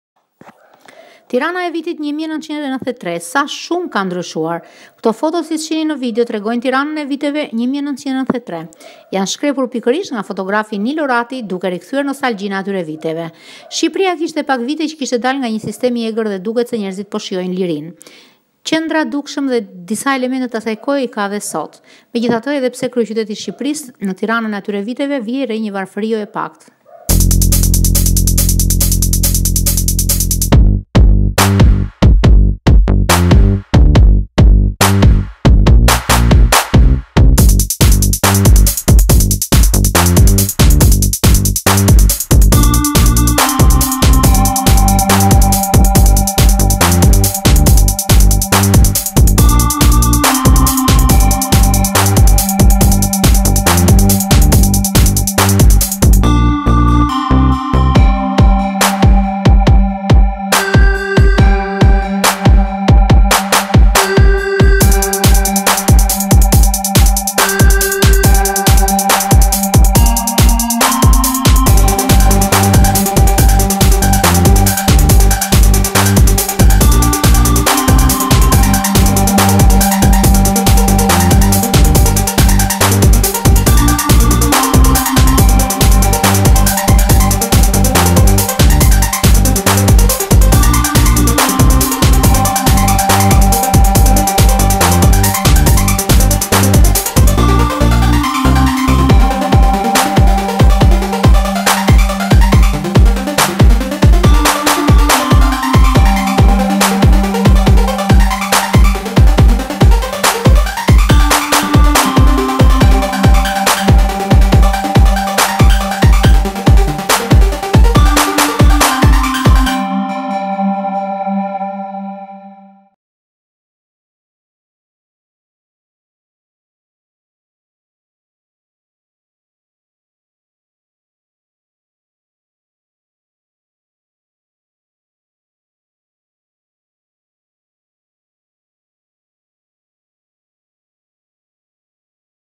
Tirana e vitit 1993, sa shumë ka ndryshuar
Këto foto tregojnë Tiranën e viteve 1993.

Janë shkrepur nga fotografi Nilo Ratti, duke rikthyer nostalgjinë e atyre viteve.

Shqipëria kishte pak vite që kishte dalë nga një sistem i egër, dhe duket se njerëzit po shijonin “lirinë”